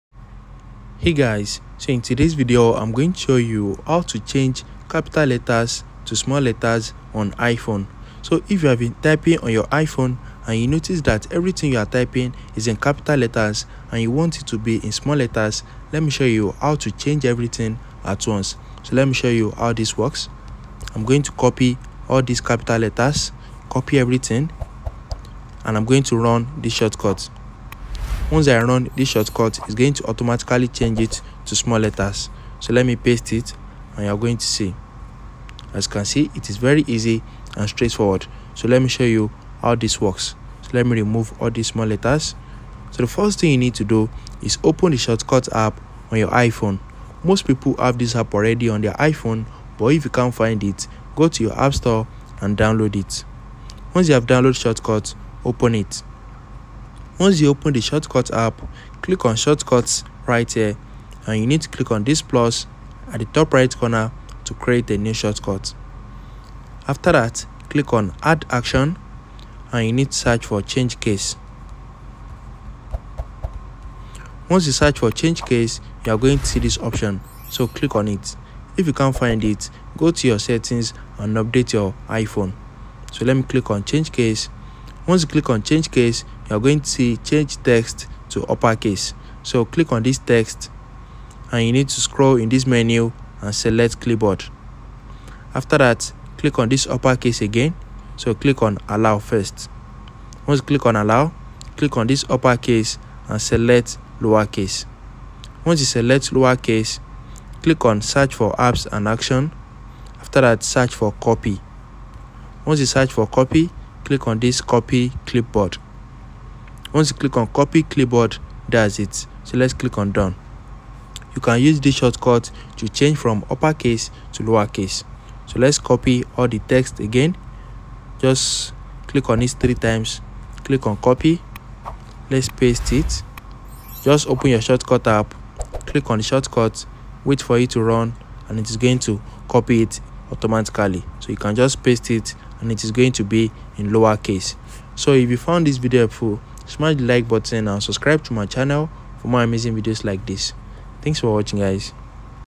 How to Change Capital Letters to Small Letters on iPhone
Discover how to effortlessly change capital letters to small letters on your iPhone with our comprehensive guide! Whether you're adjusting the formatting of a document or prefer the readability of lowercase letters, this tutorial has you covered. Learn simple methods to convert text to small letters across various apps and settings on your iPhone. Watch now and take control of your text formatting with lowercase letters!